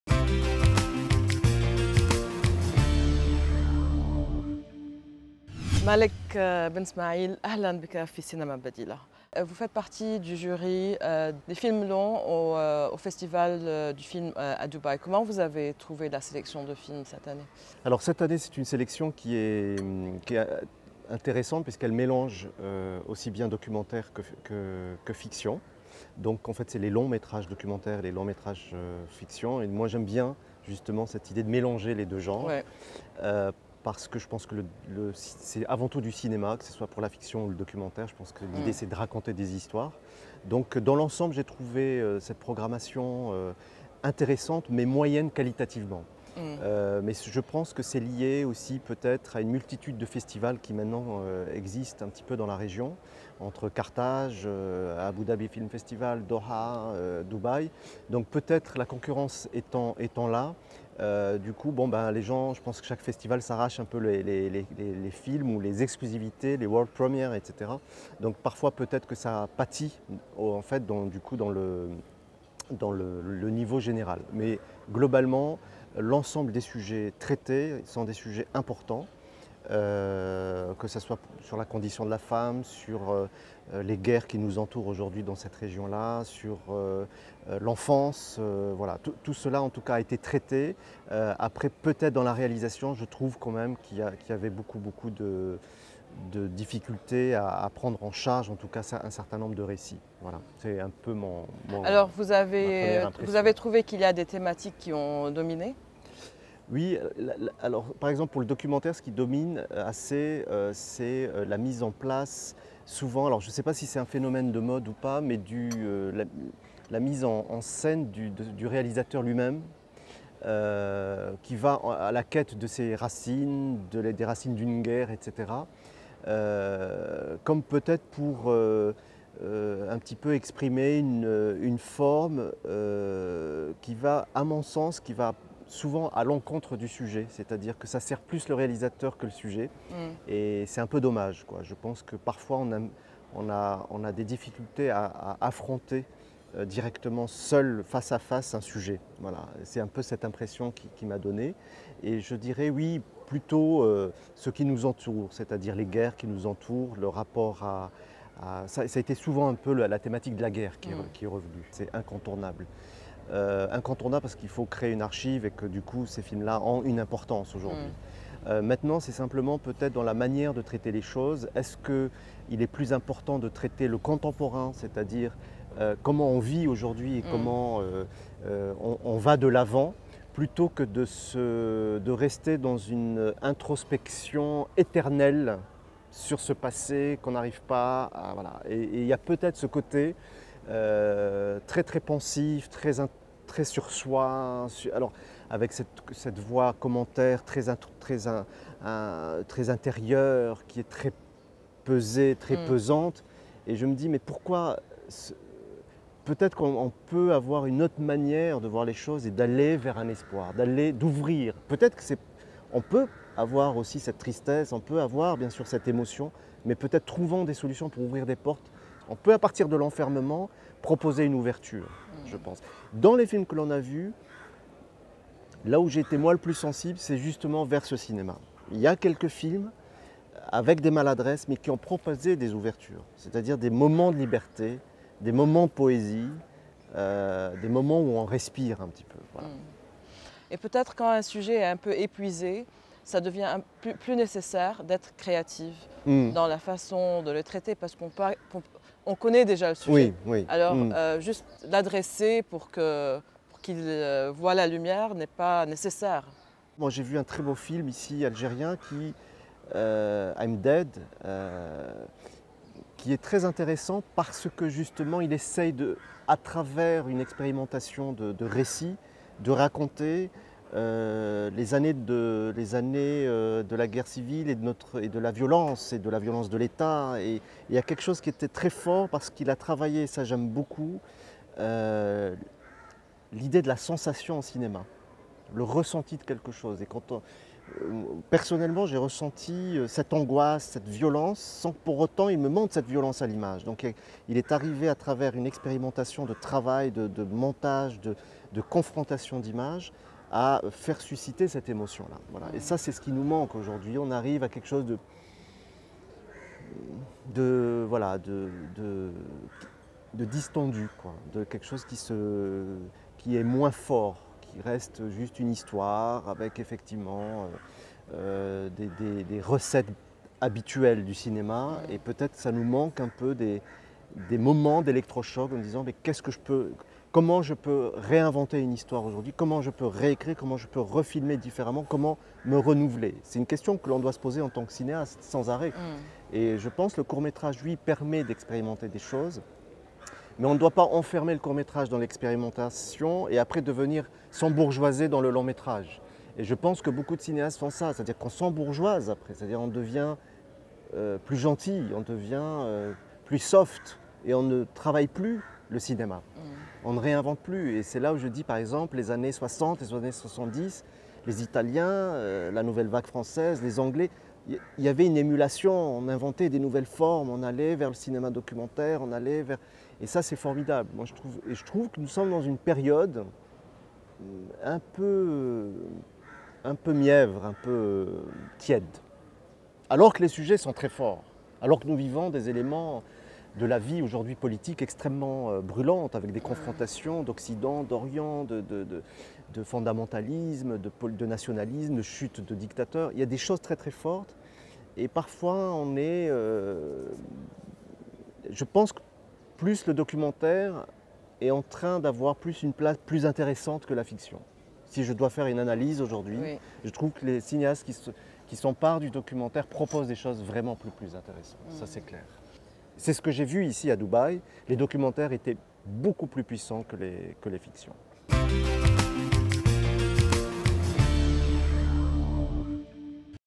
سينما بديلة Cinema Badila - Interview with Malek Ben Ismael
المخرج مالك بن اسماعيل كان عضو أحدى لجان مهرجان دبي السينمائي ٢٠١٤ قابل سينما بديلة وتحدث عن خاصيات ونماذج الأفلام العربية
Dubai Film Festival committee member Malek Ben Ismael talks to Cinema Badila about the film industry.